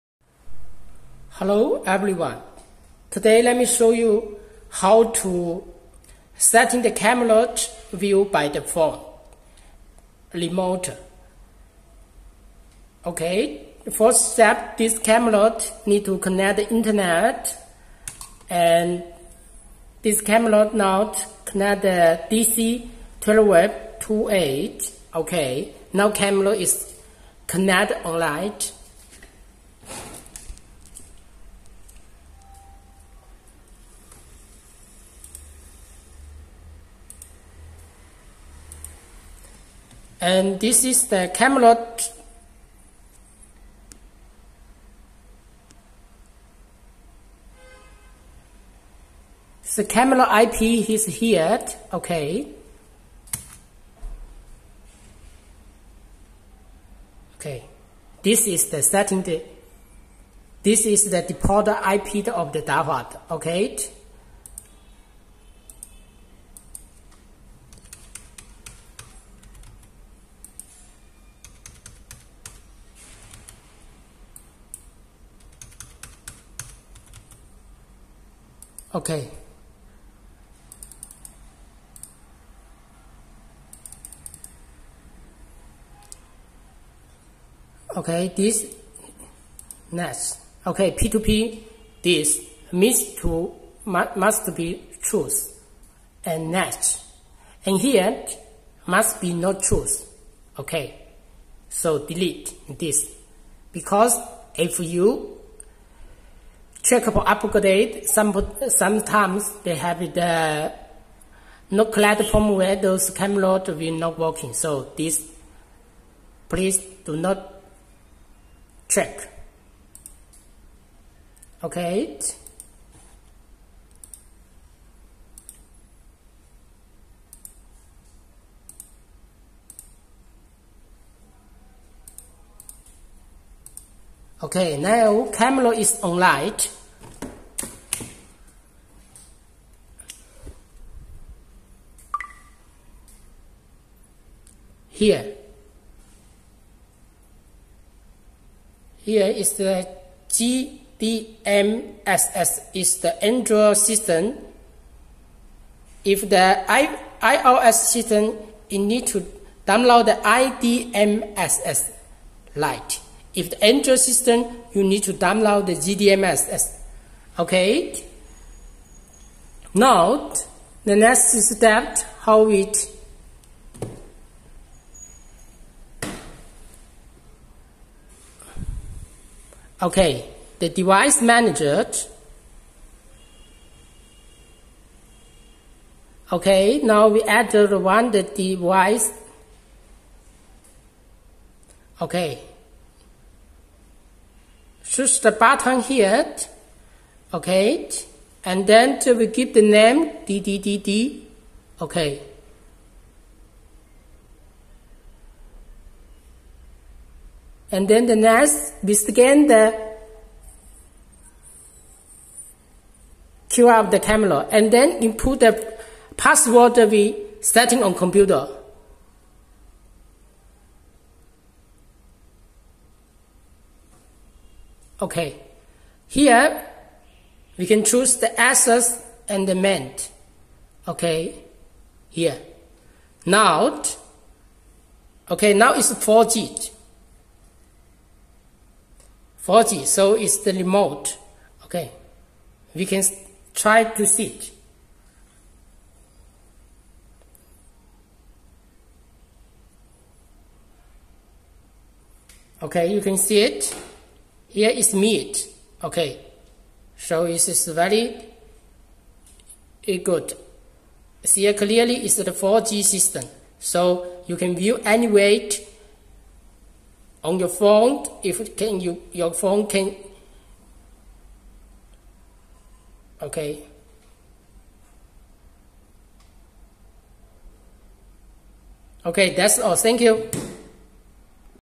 how to view dahua IP camera from phone remote
1： download  gdmss lite  （Android ）idmss lite （IOS）
2: connect camera  on route  setting camera password （remember choose P2P but not choose update check on line ）

3 : open GDMSS → Device Manager → wired Device→ P2P→  and input name （setting by your self ）→ SN ( can scan QR code or input SN no )→  Username (admin) → password ( setting  on computer )